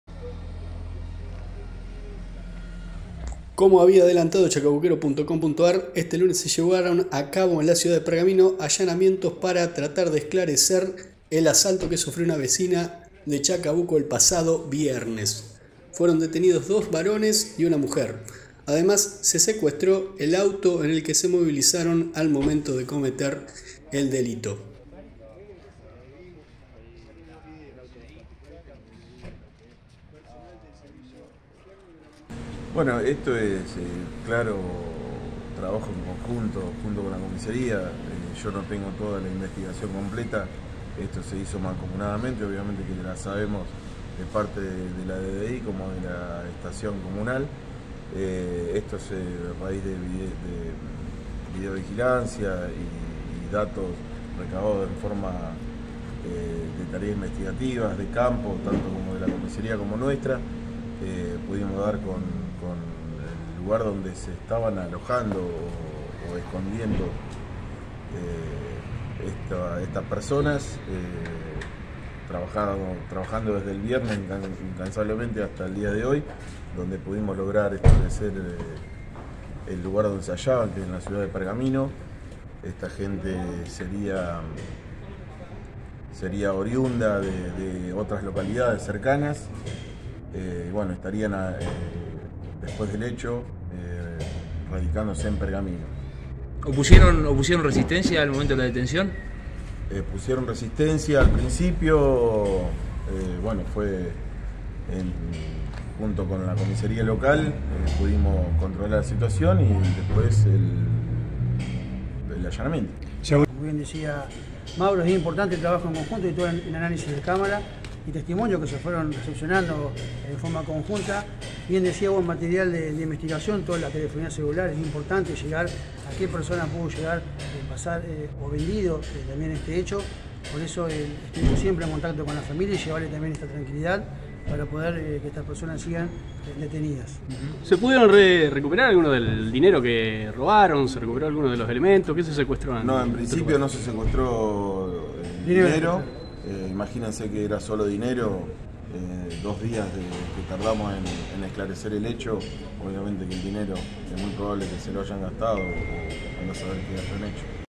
Así trasladaron a los detenidos por el robo a una vecina
Capítulos:

00:00 Tres detenidos
00:32 Habla DDI
01:35 ¿Opusieron resistencia?
01:52 Habla Jefe de Policía
02:16 ¿Se pudo recuperar el dinero?